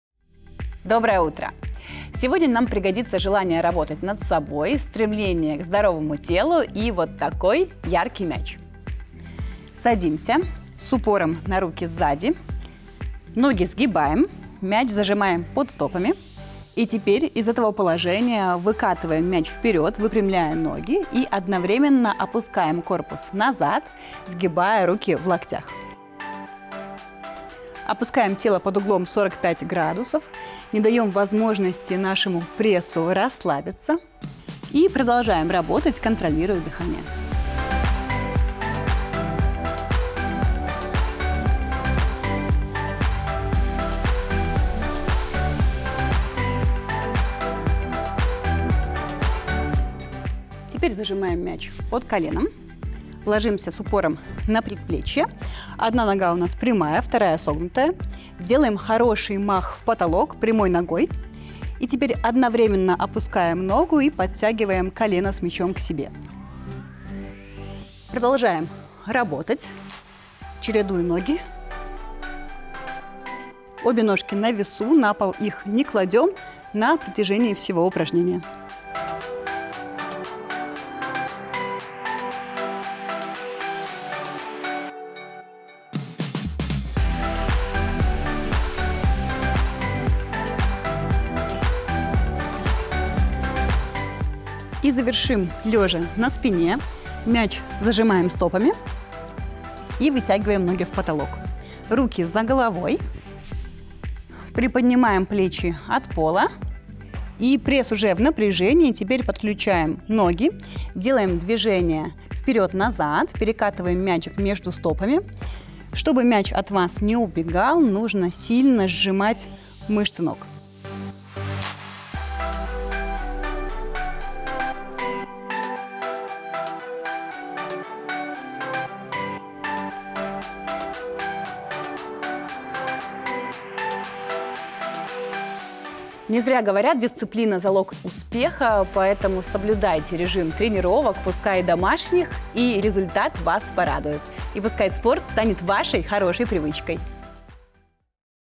ЗАРЯДКА с МЯЧОМ – простые упражнения | Утренняя зарядка
Стремление к идеальному телу и упругий мяч – что ещё нужно для качественной зарядки? Сегодня Ксения Санкович покажет несколько простых и эффективных упражнений.